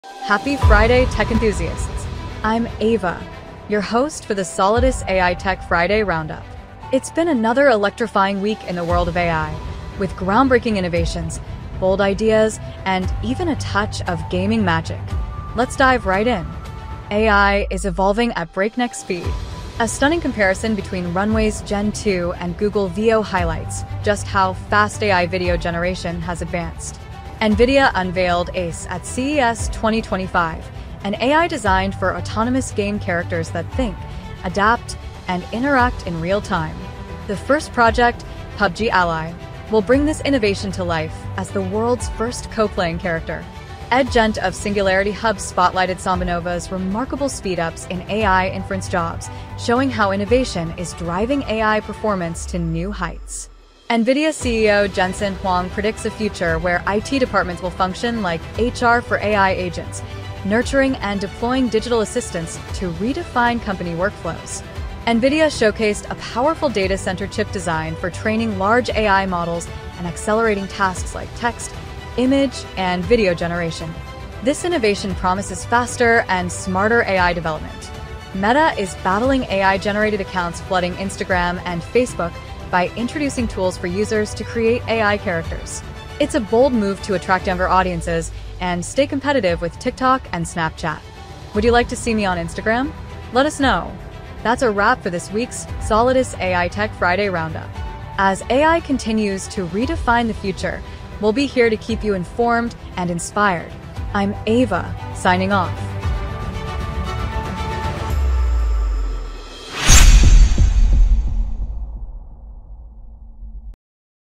Frid-AI Roundup with AVA | January 10
🗞️ Ava's Frid-AI Roundup!

Welcome to Solidus Ai Tech Friday Roundup!

✨ SambaNova’s AI Breakthrough
⚡ The Pace of AI Innovation
🌐 NVIDIA’s Game-Changing ACE
🤖 AI Agents as the Future of IT
⚙️ NVIDIA’s GB200 NVL72 Chip
🛡️ Meta’s Fight Against AI Spam